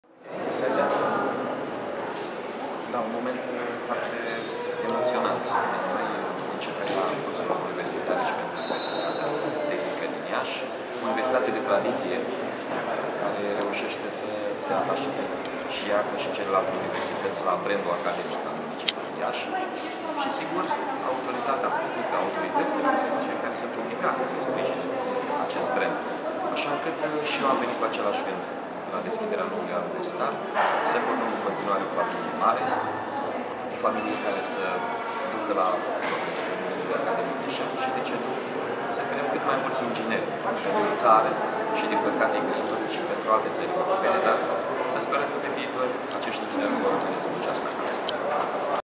Ceremonia de deschidere a anului universitar 2017 - 2018 la TUIASI
Ceremonia de deschidere a anului universitar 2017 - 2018 la Universitatea Tehnică „Gheorghe Asachi” din Iași